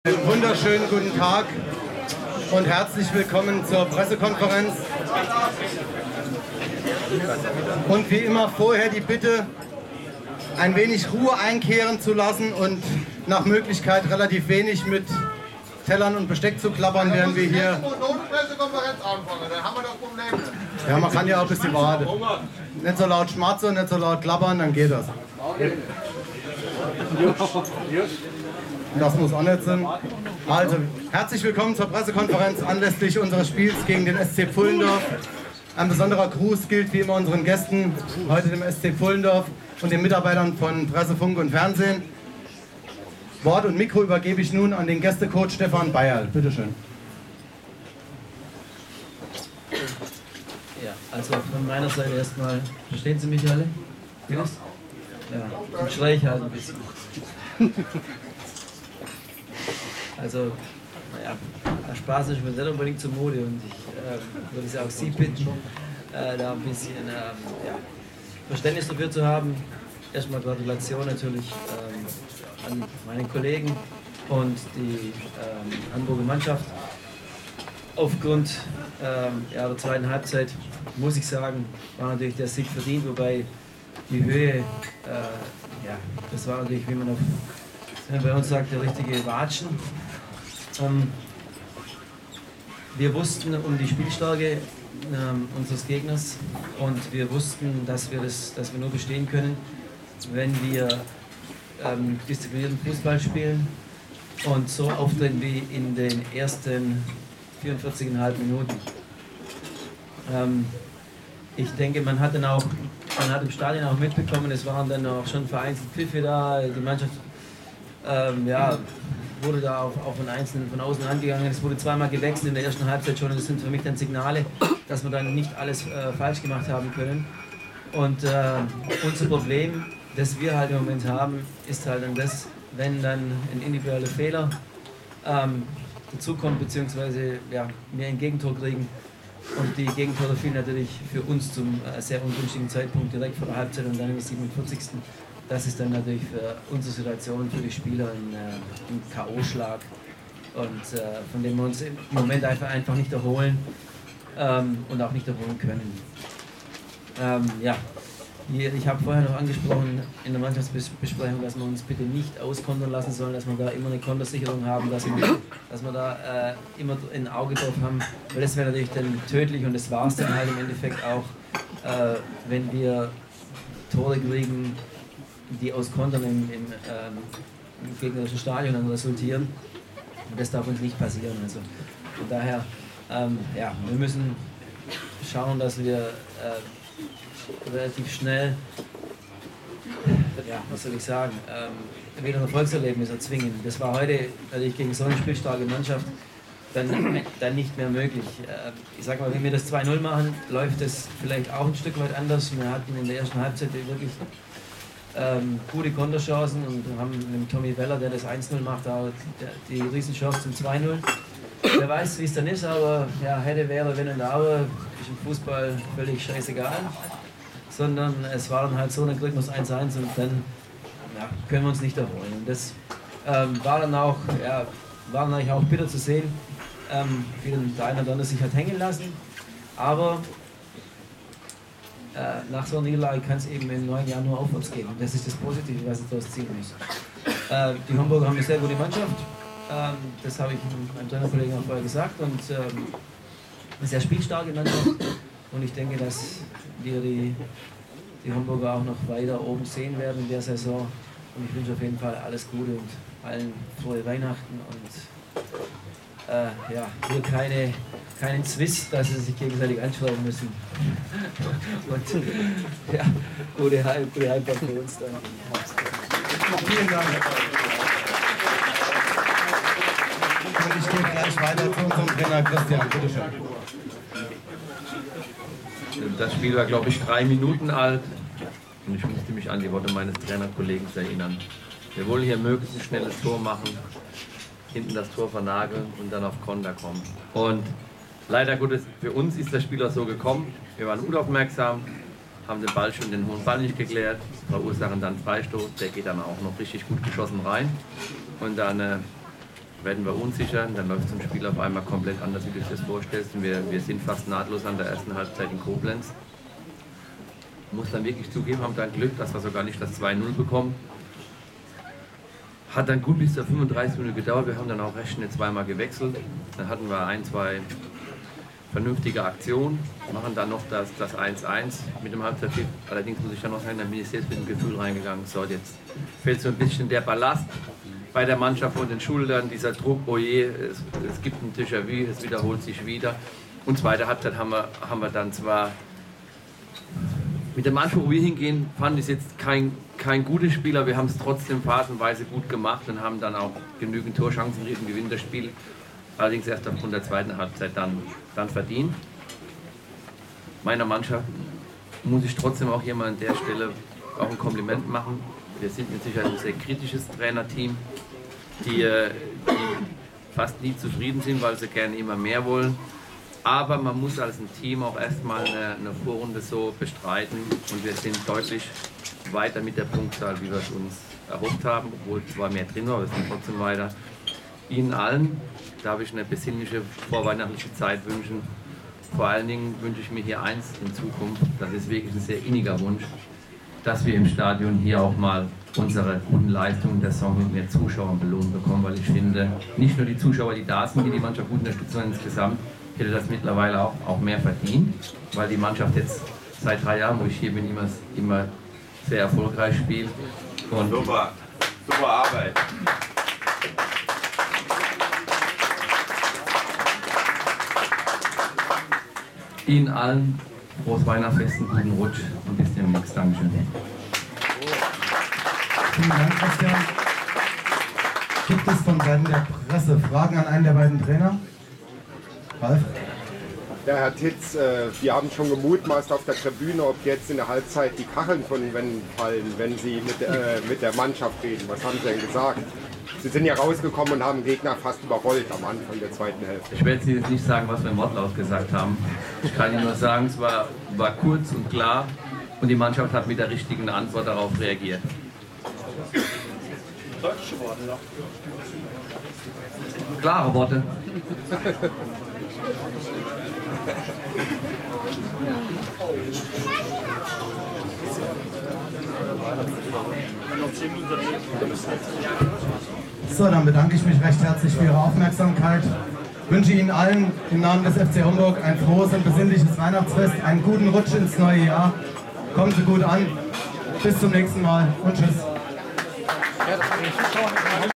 Pressekonferenz FC Homburg - SC Pfullendorf
08.12.13 - Pressekonferenz FC Homburg - SC Pfullendorf. Endstand: 6;1 (1:1)